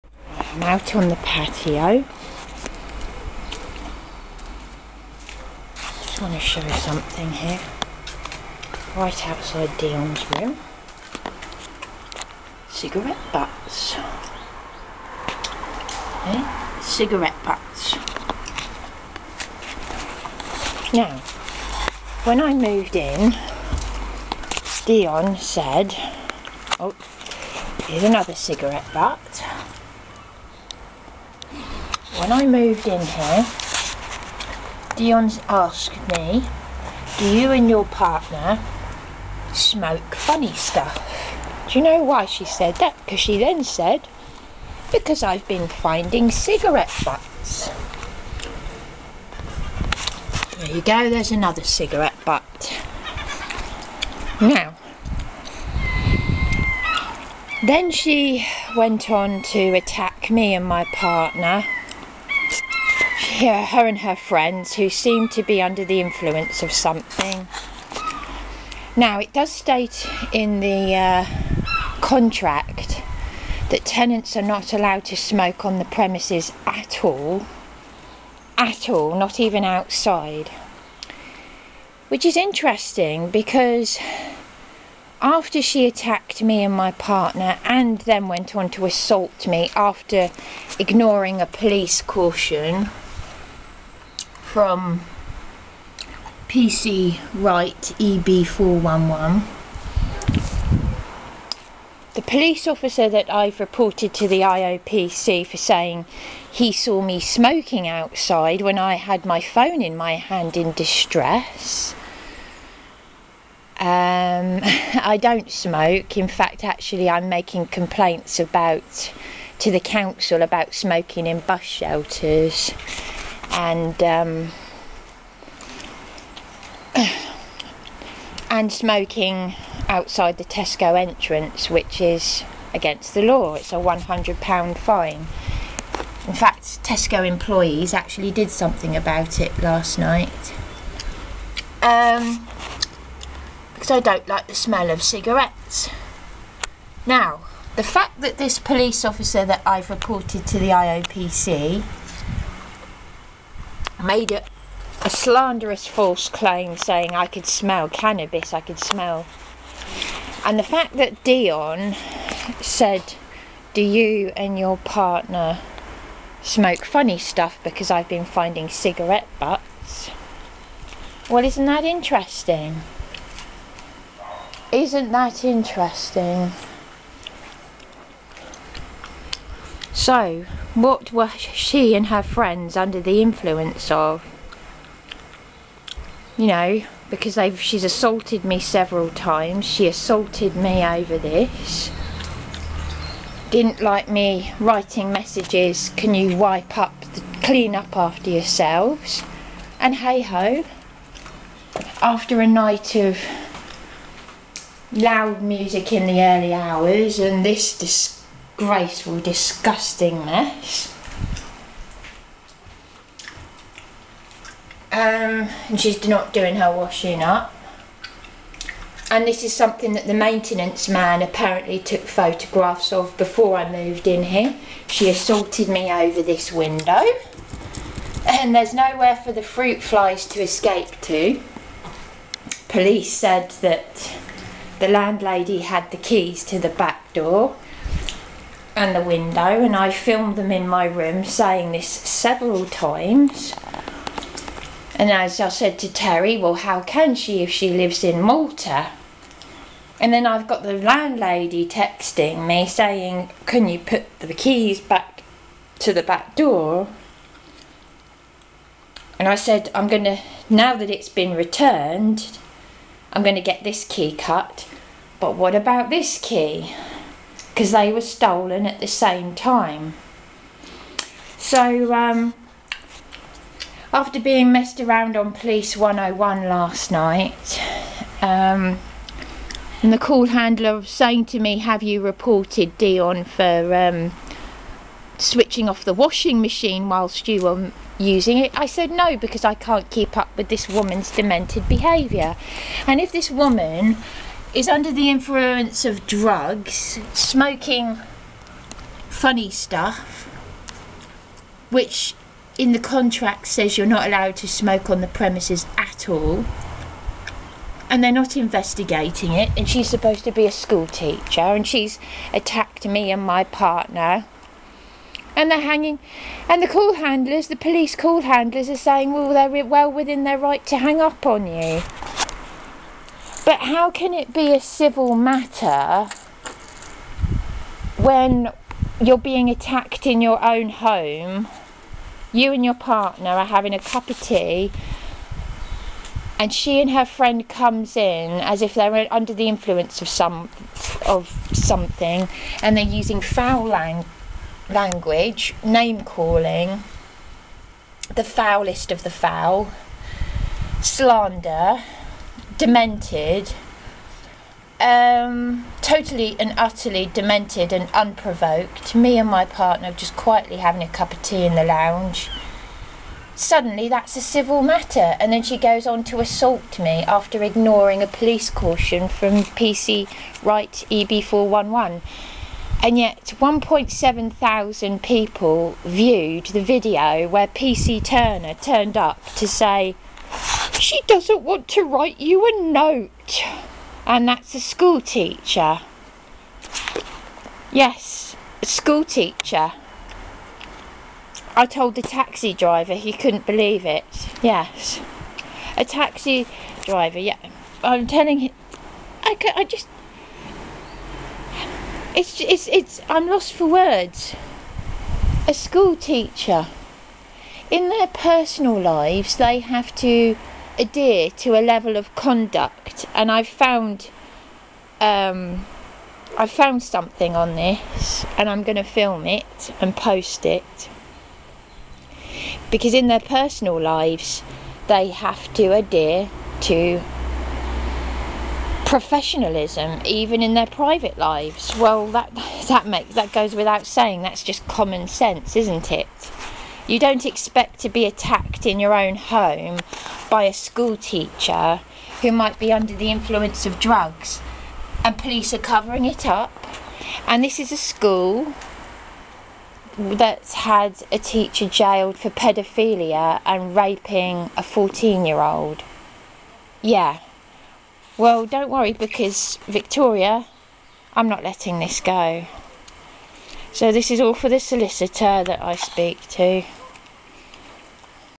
*EVIDENCE OF CIGARETTE BUTTS IN COURTYARD PATIO (BREACH OF CONTRACT) SEE DESCRIPTION!!!
*IT IS A BREACH OF CONTRACT TO SMOKE ANYWHERE ON THE GROUNDS OF THE PROPERTY BUT THE FACT THAT I COULD SMELL CANNABIS THE OTHER NIGHT IN MY ROOM AND A POLICE OFFICER WHOM I REPORTED TO THE IOPC FOR PATENTLY LYING SAYING HE COULD SEE ME SMOKING CANNABIS WHEN I DETEST SMOKING SO MUCH I HAVE COMPLAINTS TO MAKE TO THE COUNCIL ABOUT ILLEGAL SMOKING IN BUS SHELTERS WHICH INCUR FINES OF £100 AND OUTSIDE TESCO ENTRANCE (A POLICE OFFICER WHO ONCE BEAT ME AFTER A DOMESTIC INCIDENT I REPORTED.) AND THE FACT THAT TENANT DIONNE ASKED ME IF ME AND MY PARTNER SMOKE FUNNY SUBSTANCES SHORTLY AFTER I MOVED IN, SAYING SHE FOUND CIGARRETTE BUTTS AND SUDDENLY I AM FINDING THEM AFTER ALL THIS HORRIFIC ABUSE; MEANS THIS WAS ALL ORCHESTRATED BY THIS DANGEROUS LUNATIC WHO IS SAID TO BE AN ART SCHOOL TEACHER AT RATTON SCHOOL.*